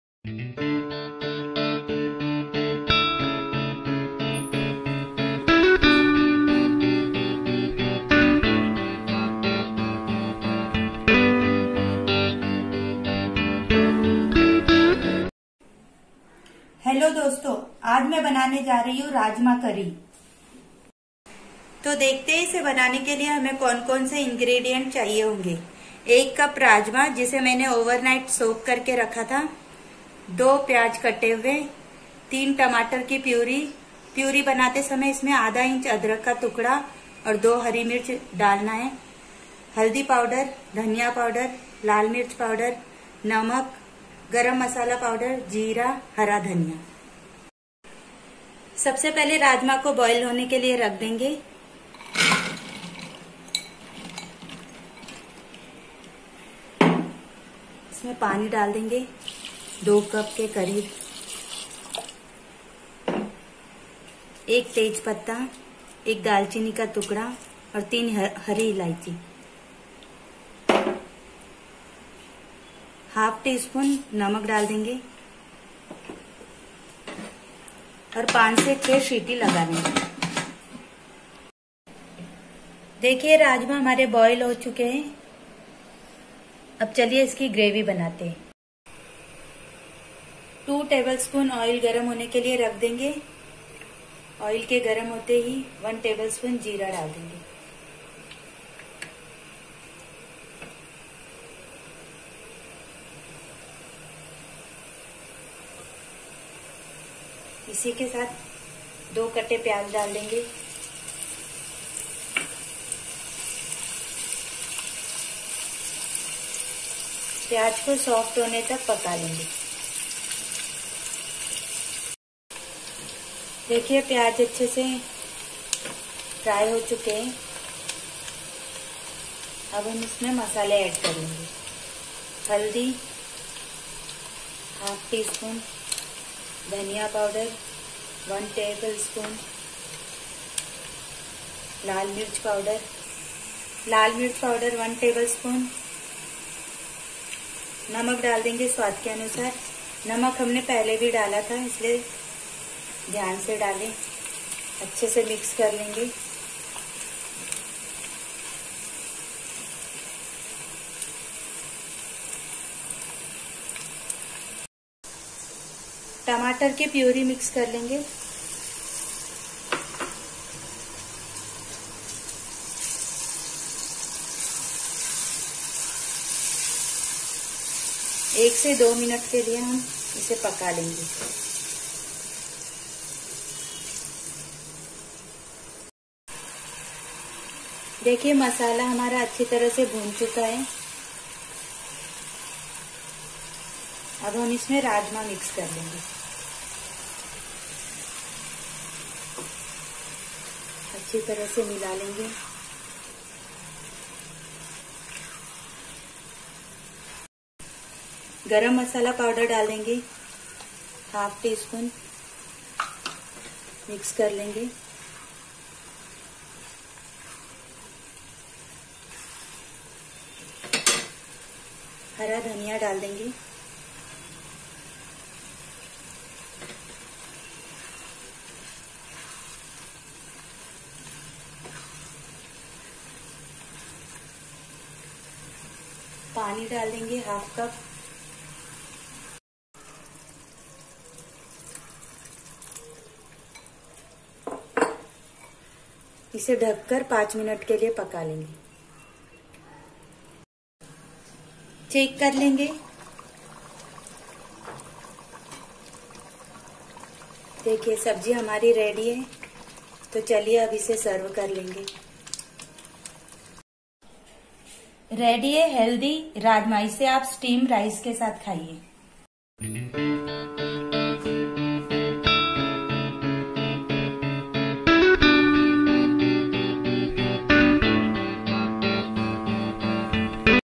Rajma Masala | Rajma Curry | Punjabi Style Rajma Recipe
Ingredients:
1.Rajma (Kidney Beans)
2.Onion
3.Tomato Puree
4.Red Chilli Powder
5.Coriander Powder
6.Turmeric Powder
7.Garam Masala Powder
8.Salt
9.Coriander Leaves
10.Oil
11.Jeera

Procedure:
Take a pressure cooker, Add soaked Rajma, Tej Patta, Cardamom, Dalchini, Salt and give 5 to 6 whistle.
Heat a pan, Add 2 tablespoon oil and add jeera and onion  saute it. Add Red chilli powder, Coriander powder, Turmeric powder. Cook it fot 2-3 mins.
Add Tomato puree and mix it well. Cover for 2-3 mins. Now add boiled rajma, half tablespoon garam masala, Salt as per the taste.
Mix it properly and cover the pan. Cook for 5 mins.

Ready to serve rajma curry.